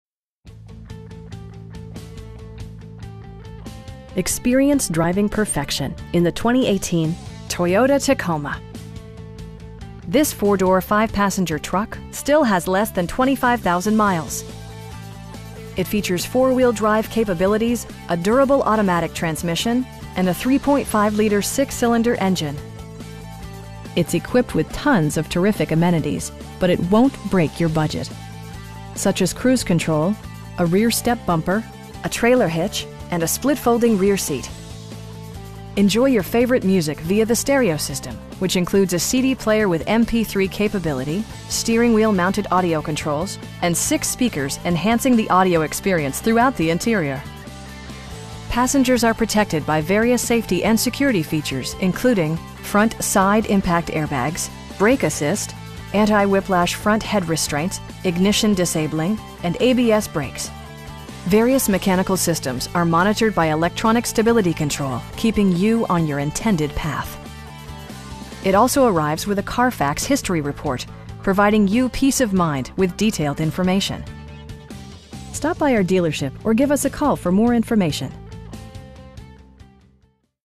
2018 Toyota Tacoma SR V6 in Missoula, MT 59804
Lithia Toyota of Missoula
4001 Brooks Street
in Missoula, MT 59804

Experience driving perfection in the 2018 Toyota Tacoma. This 4 door, 5 passenger truck still has less than 25,000 miles! It features four-wheel drive capabilities, a durable automatic transmission, and a 3.5 liter 6 cylinder engine. It's equipped with tons of terrific amenities, but it won't break your budget. Such as cruise control, a rear step bumper, an outside temperature display, front bucket seats, power door mirrors, power windows, a trailer hitch, and a split folding rear seat. Enjoy your favorite music via the stereo system, which includes a CD player with MP3 capability, steering wheel mounted audio controls, and 6 speakers, enhancing the audio experience throughout the interior. Passengers are protected by various safety and security features, including: head curtain airbags, front side impact airbags, traction control, brake assist, anti-whiplash front head restraints, ignition disabling, and ABS brakes. Various mechanical systems are monitored by electronic stability control, keeping you on your intended path. It also arrives with a Carfax history report, providing you peace of mind with detailed information. We pride ourselves in consistently exceeding our customer's expectations. Stop by our dealership or give us a call for more information.